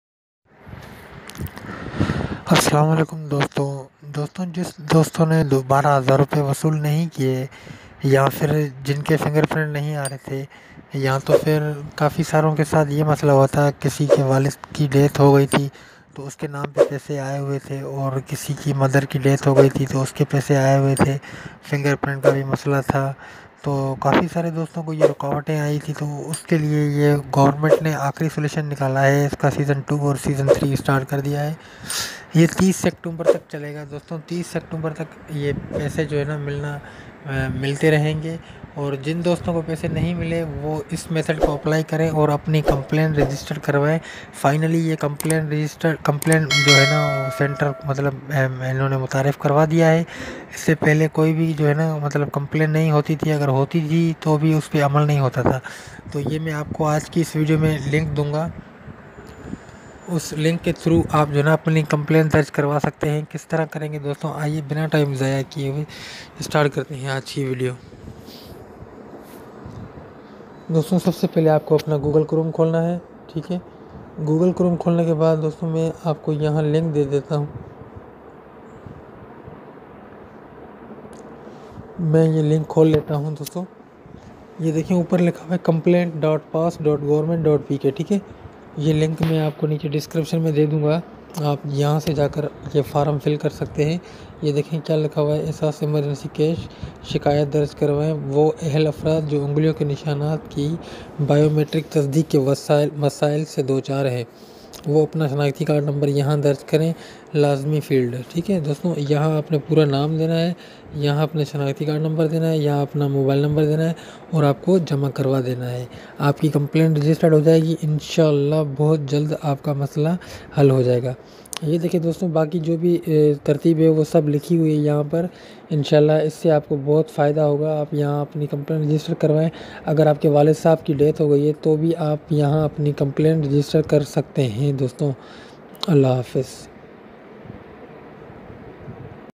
Ehsaas Emergency Cash Program Session 2 Start Ho Chuka He Jis Ne 24000 Nahi Liye Wo Apply Kare
Asslam alelum dosto aaj ki is video me mene ap logo ko ye bataya he ke jis dost ne ehsaas emergency program ke tehat 24000 nahi liye wo aaj apply kare or session 2 me apni registration laazmi karwai jis ke fingerprint nahi aate wo apni complain bhi registered karwa sakta he is portal me dosto link ap ko neche de diya he